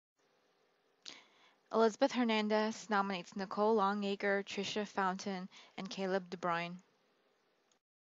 Fancy Dress Challenge - Elizabeth Hernandez
You've heard of the 'Ice Bucket Challenge'? Well in aid of the Bright Horizons Foundation for Children, staff across Bright Horizons are taking part in the Fancy Dress Challenge.